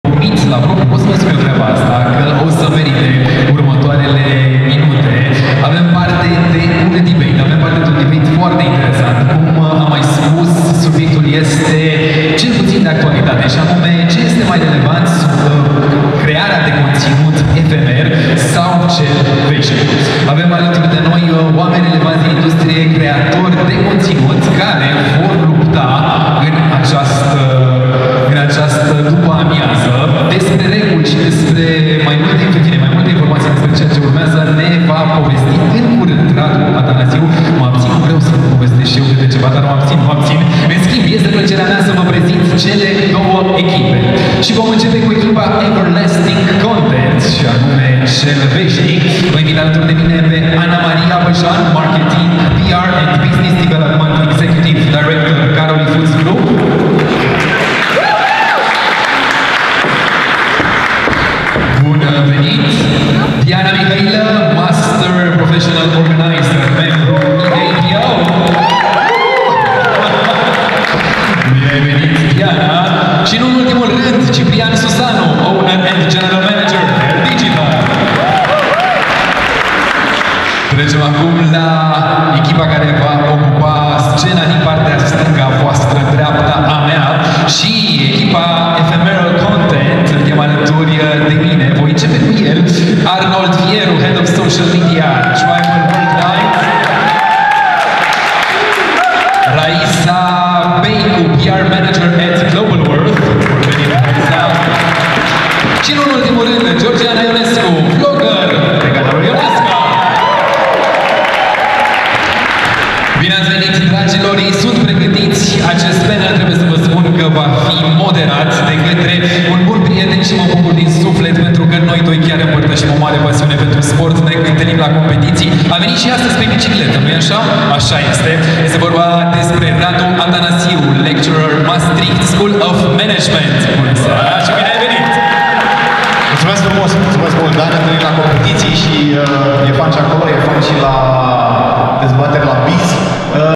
(21) Alex Grecu - Moderare - Social Media Summit București 2020 - Biz (2020.03.05, Palatul CEC)
În data de 5 martie 2020, a vut loc la Palatul CEC "Social Media Summit București 2020" - eveniment marca Biz Agency.